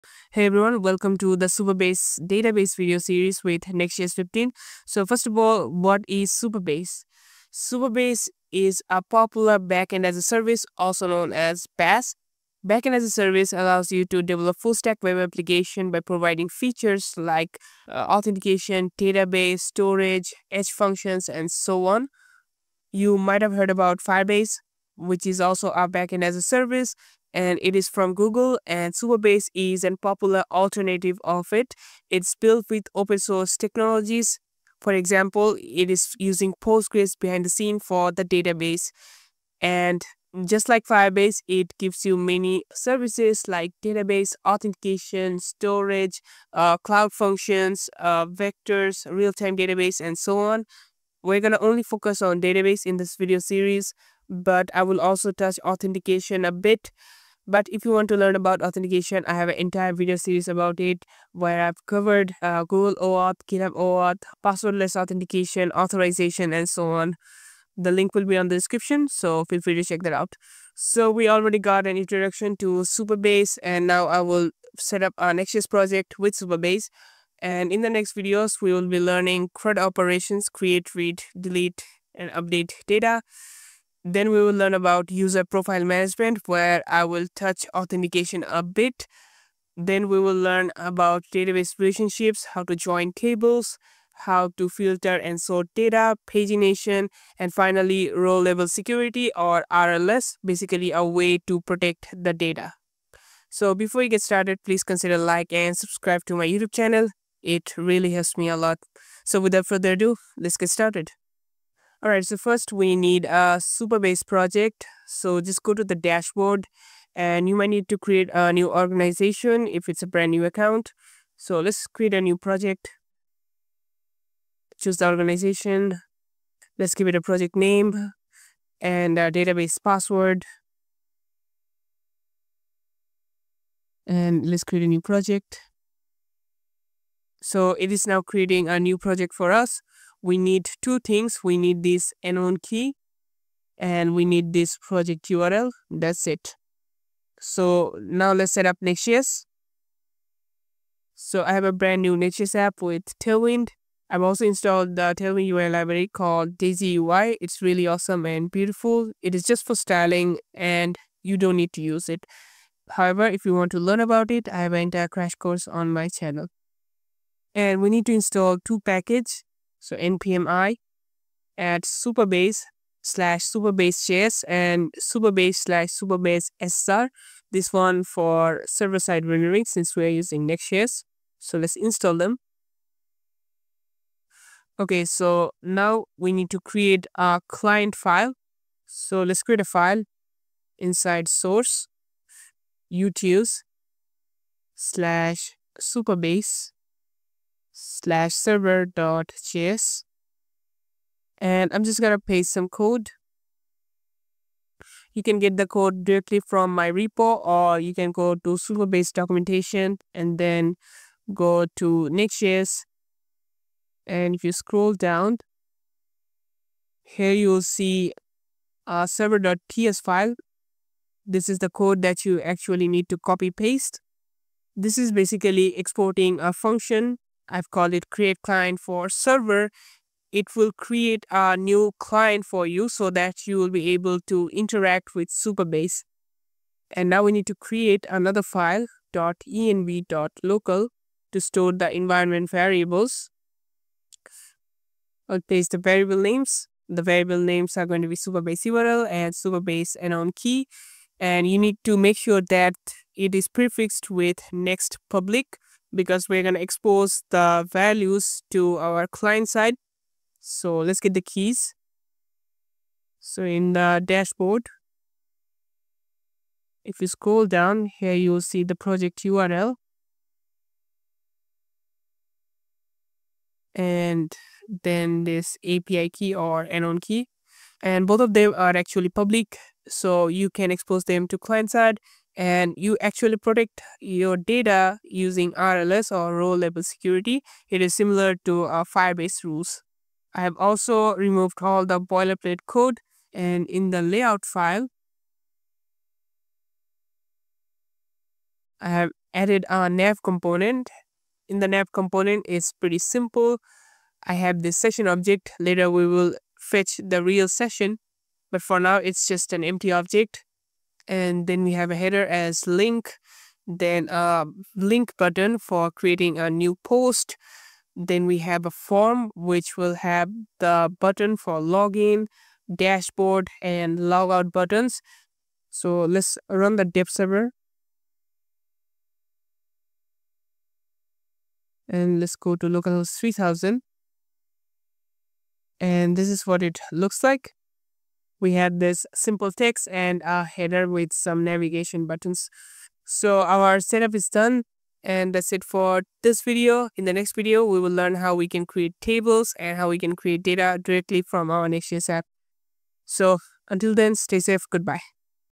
1. Setup Supabase Database with Nextjs 15 | Server Component | Server Action | React
Introducing you to Supabase Database with Nextjs 15, where we will understand what Supabase is and why we need it. Also, how to set up nextjs with superbase.

Timestamp:
0:00 What is Supabase & why do we need Supabase?
1:50 Setup supabase project
2:30 Setup nextjs with supabase

Stay safe and good bye.